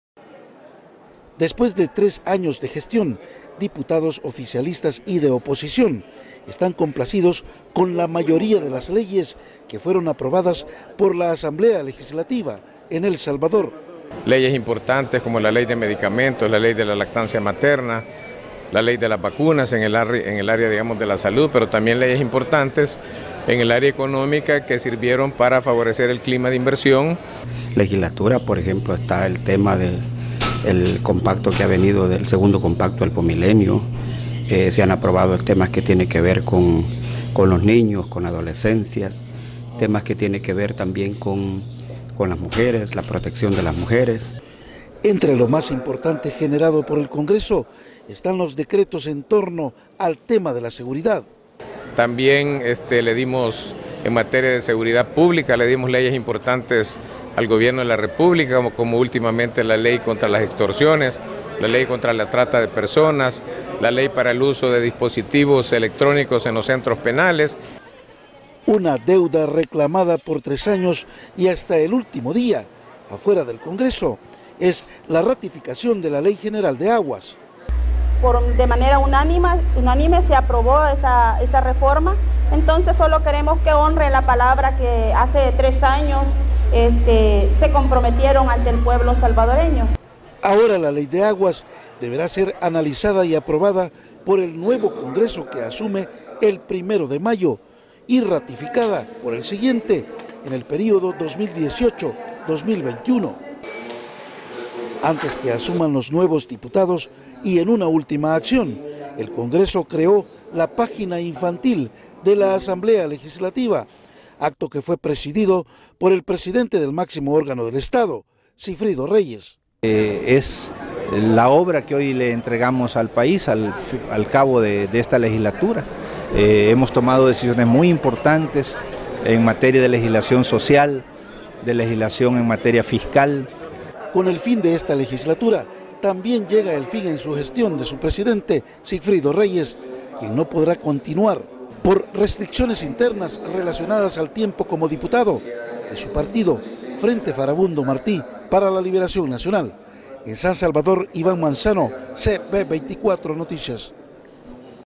Congreso de El Salvador terminará periodo de gestión dejando pendiente ley de aguas
La Asamblea Legislativa de El  Salvador llega al final de tres años de gestión,  en los que,  según el oficialismo,  se han logrado aprobar leyes a favor de los más desposeídos,  quedando deudas como la Ley General de Aguas,  en busca de un Congreso más transparente y cercano a la gente.